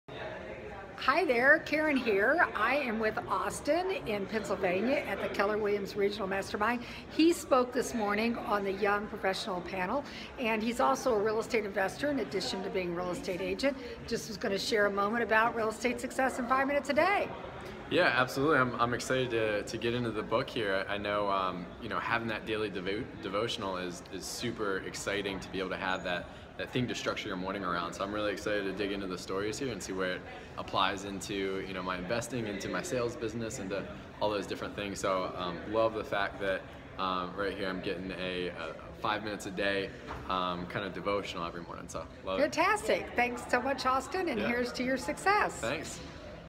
Austin Carrol - 5 Minute Success Stories
Austin Carrol of the Menkiti Group share his thoughts and success with Real Estate Success in 5 Minutes a Day!